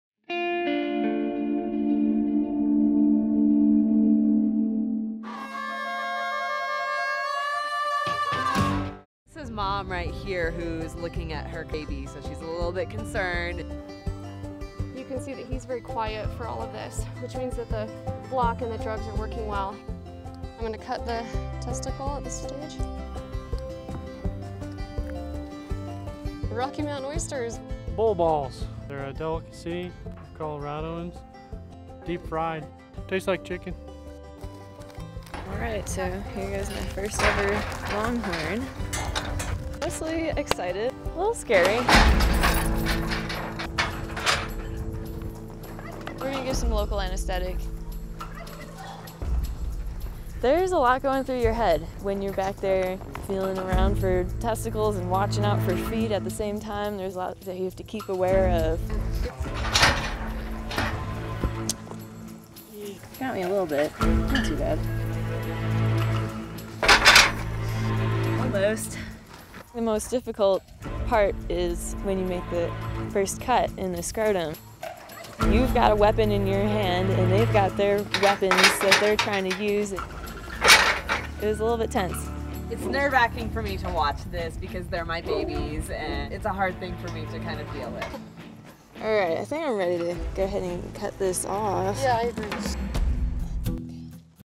Getting Rocky Mountain Oysters the Hard Way
Dr. Amy Hutcheson is on a Colorado ranch performing castrations on young bulls. Being behind the powerful animals in kicking range is an anxious place to be while performing this procedure.